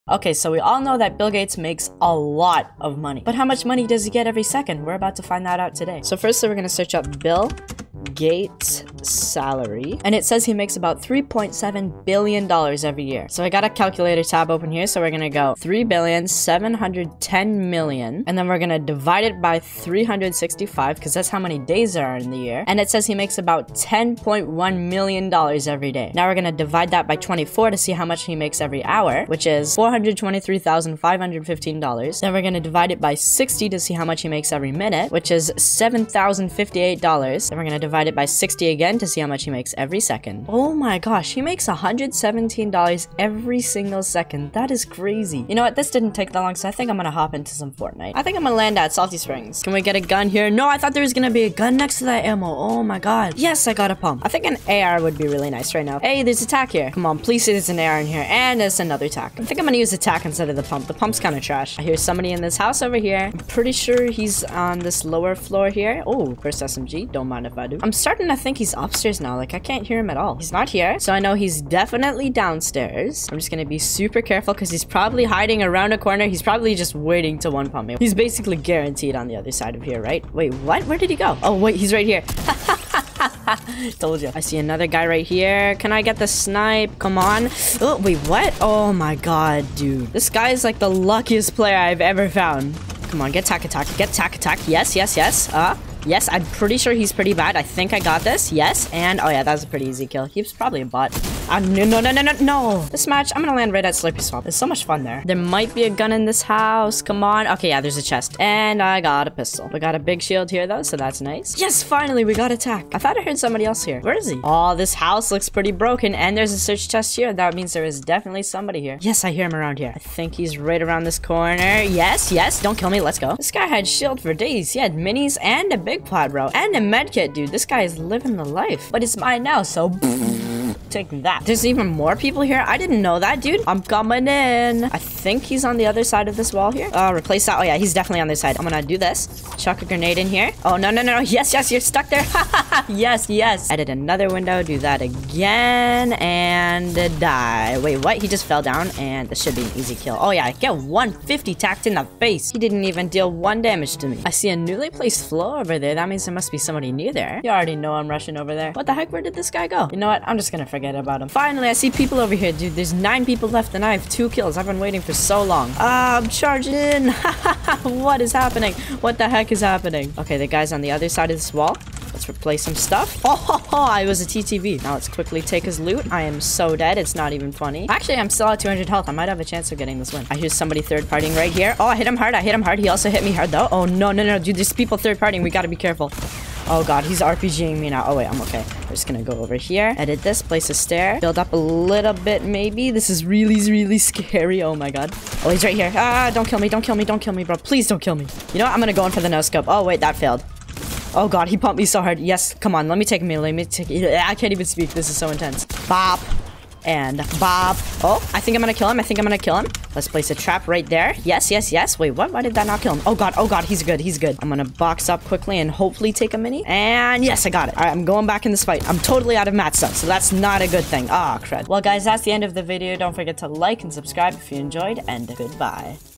How Much Money Does Bill Gates Make a Second?
In this video I did some quick calculations to figure out how much money Bill Gates makes every second.